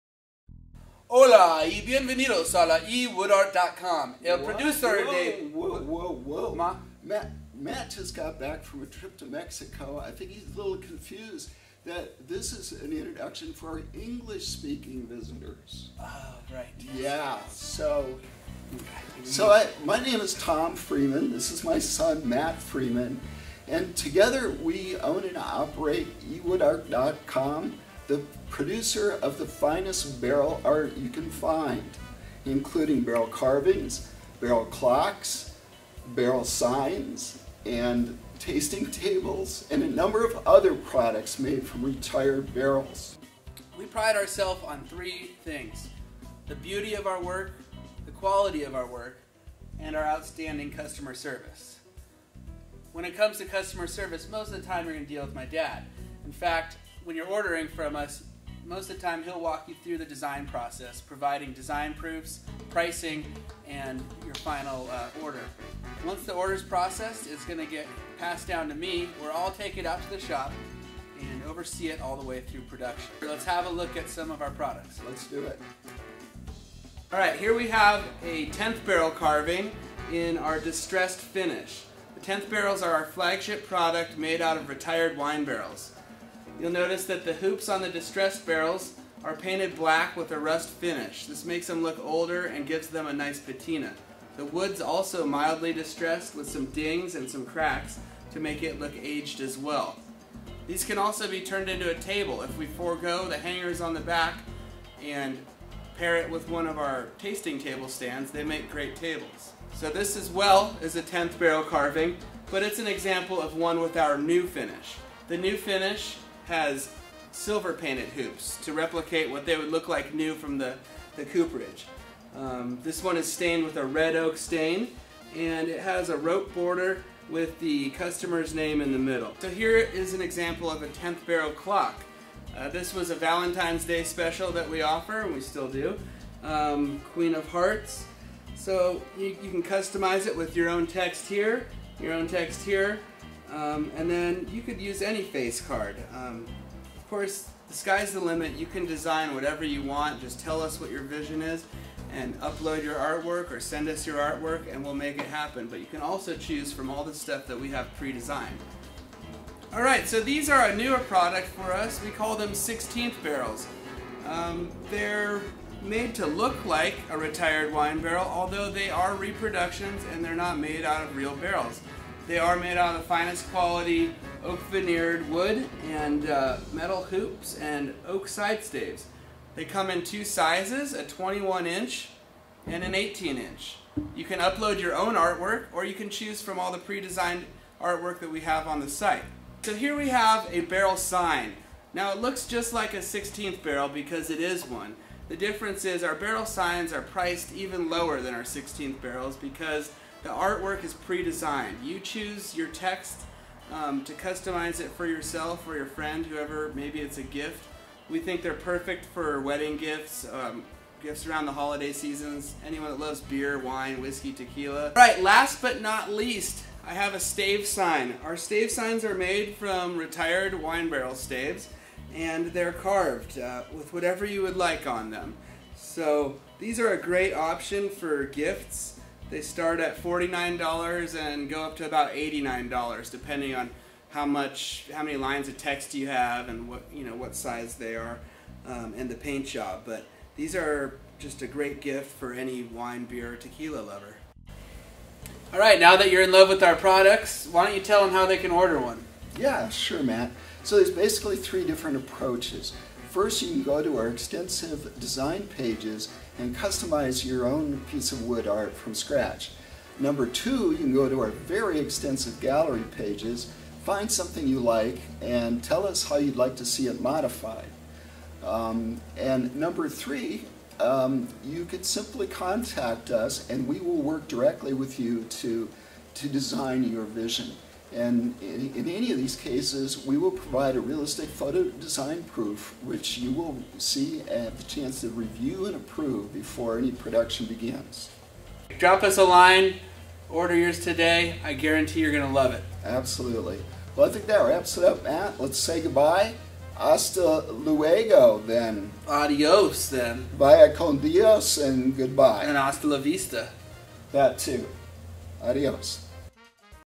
Custom Wine Barrel Art, Barrel Carvings and Signs by eWoodArt.com
Our wine barrel carvings are the perfect customizable addition to your wine cellar, bar, restaurant or home.  From barrel clocks, wine barrel tables, or barrel art carvings, you are sure to create something just as unique as yourself.  Our attention to detail shows in the beauty of these heirloom quality pieces that will become the centerpiece to your room. and customize your own personal wine barrel carving today!

We also make:
  -Wine barrel clocks
  -wine barrel tasting tables
  -Wine barrel chalk board easels
  --large reproduction wine cask heads